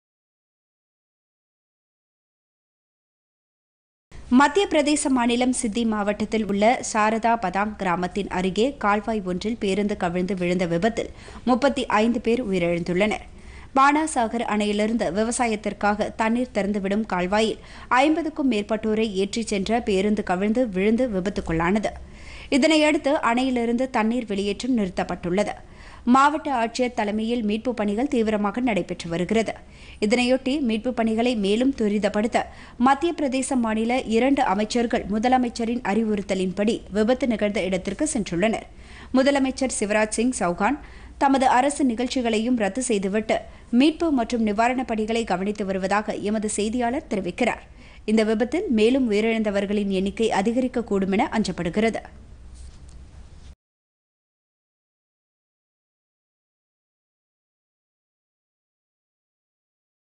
மத்தியப்பிரதேசம் - பேருந்து விபத்து
மத்தியப் பிரேசத்தில் பேருந்து கால்வாயில் கவிழ்ந்த விபத்தில் - 35-க்கும் மேற்பட்டோர் உயிரிழப்பு - மீட்பு பணிகள் தீவிரம்